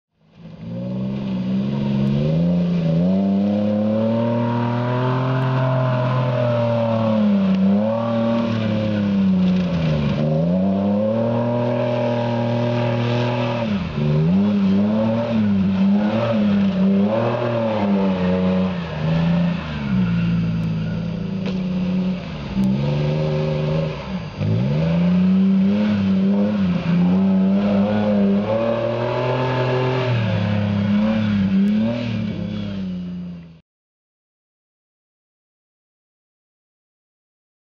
Tacoma in Ocotillo
Having some fun driving around in the sand.